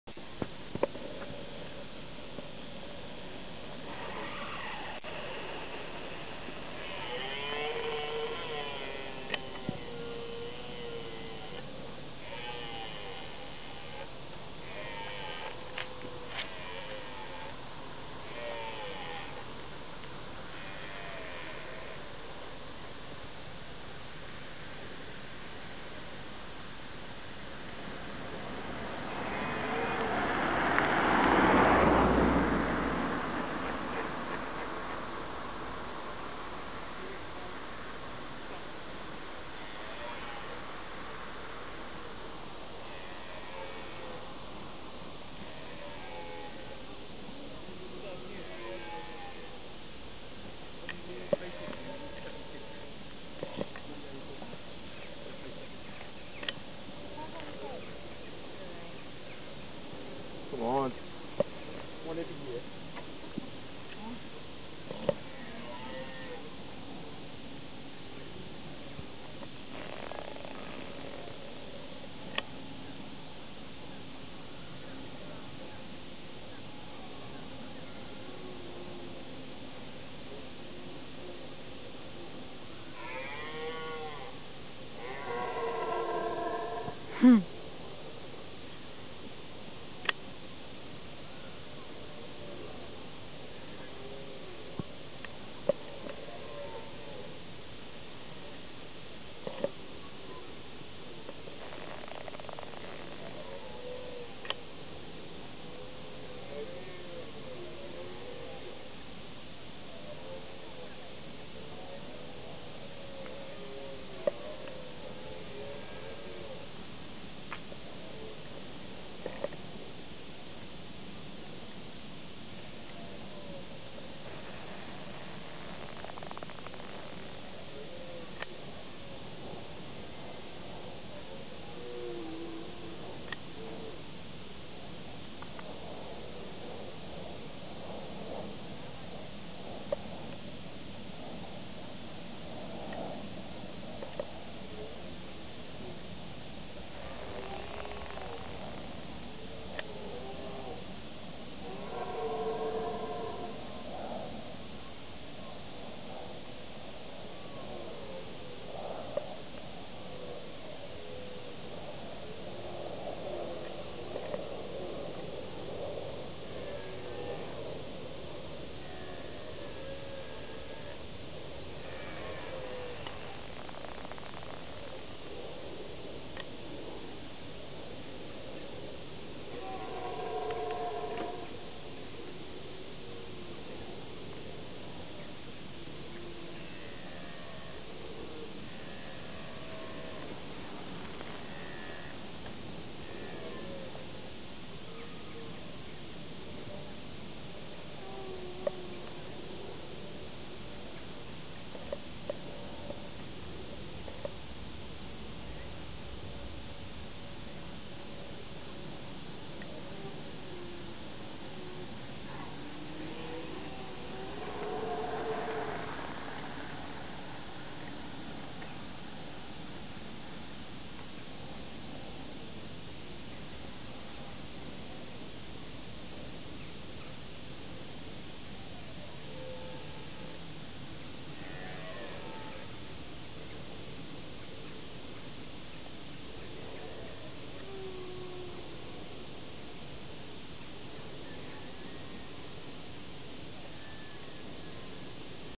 Deer in Richmond Park. The mating games..
Didn't look  like he was getting much attention...!